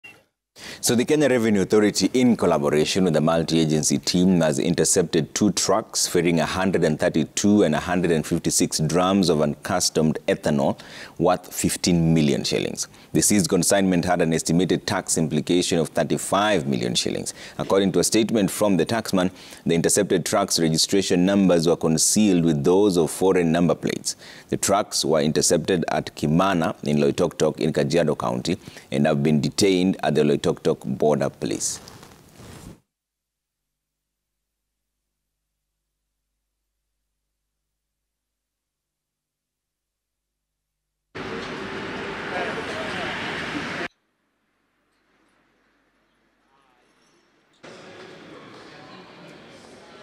KRA intercepts unaccustomed ethanol
The Kenya Revenue Authority, in collaboration with a multiagency team, has intercepted two trucks ferrying 132 and 156 drums of unaccustomed ethanol worth Ksh15million.